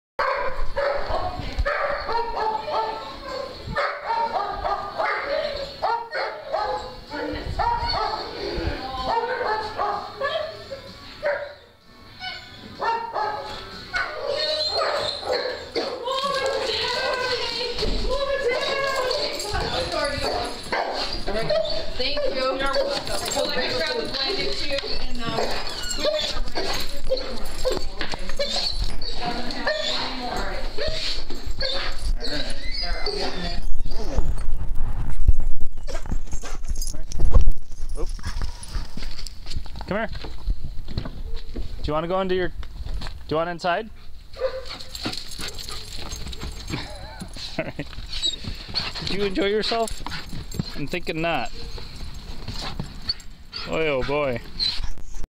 Lucy Back from the Kennel - 1
Picking up Lucy - she always hated it and this is her booking it from the kennel to my car.  Hard to track her because she is so fast.